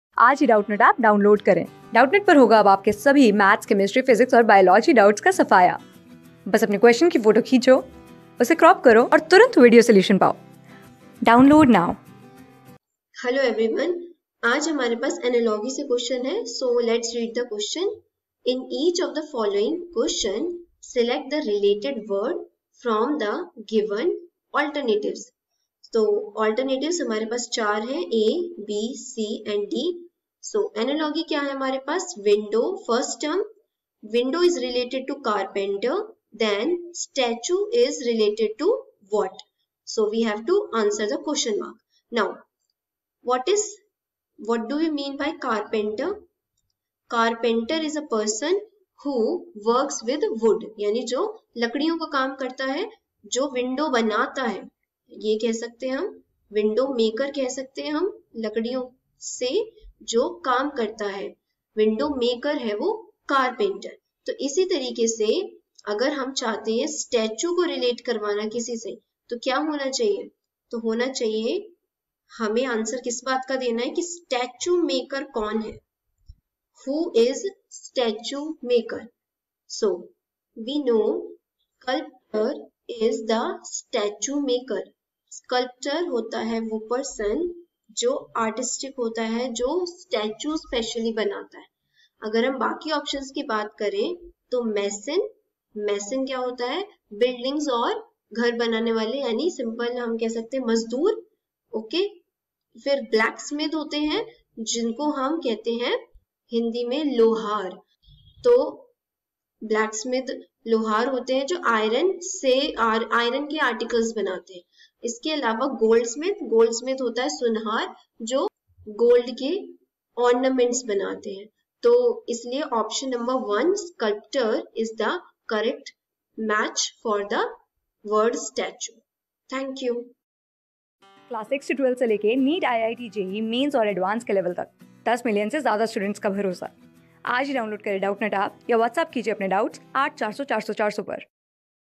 In each of the following questions, select the related word/number from the given alternatives. ...
In each of the following questions, select the related word/number from the given alternatives. Window : Carpenter :: Statue : ?
Class: 14
Subject: REASONING
Chapter: SOLVED SSC GD (2012 SHIFT-2)
Board:GOVT

👉 Have Any Query? Ask Us.